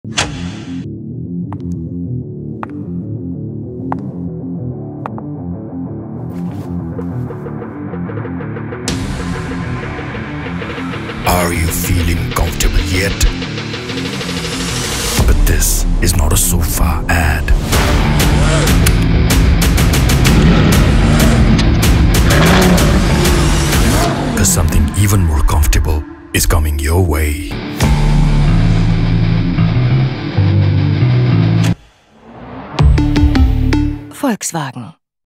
Something even more comfortable is coming your way. Stay tuned to find out.
Can you feel the comfort? Because something even more comfortable is coming your way.
Volkswagen Taigun | New SUVW | Coming Soon |

Agency: brand LogiQ
Production Partner: Jazz Production

Also join the Volkswagen Nepal Viber community to know more about Volkswagen Nepal